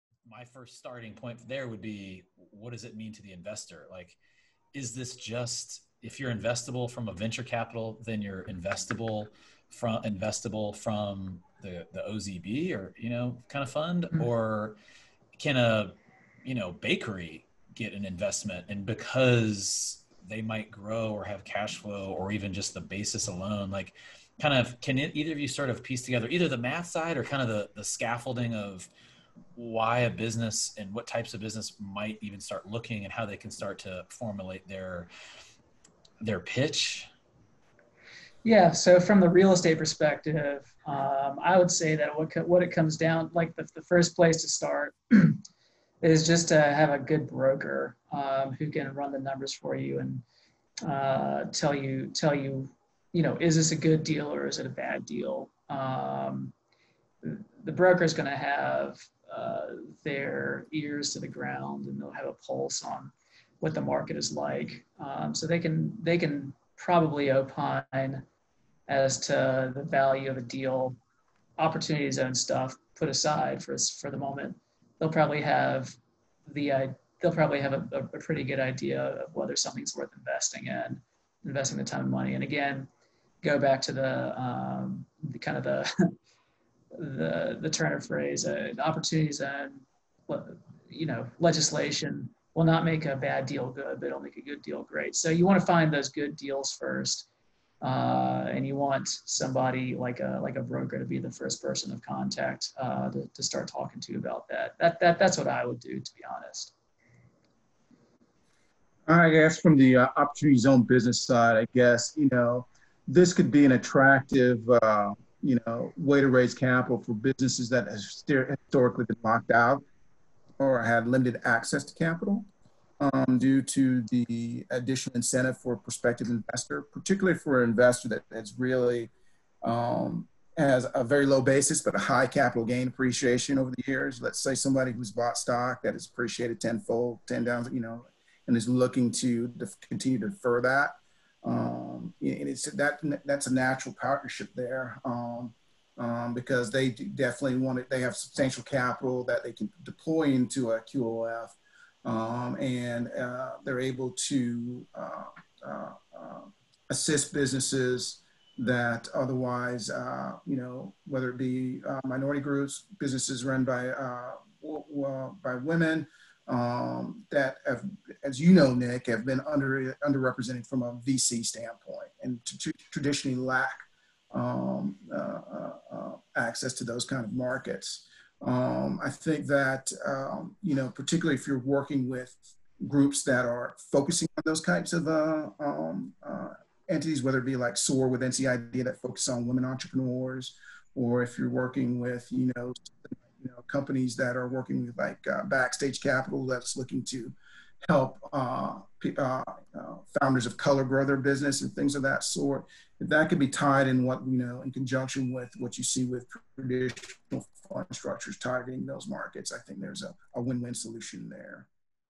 What can small businesses do to attract QOZB investments? (clip)
What can small businesses do to attract QOZB (Qualified Opportunity Zone Business) investments? What do smaller businesses need to get started as a Qualified Opportunity Zone Business?

Want to take this episode with you?
anchor.fm/nextmattersmost

We've got more to offer on our social platforms!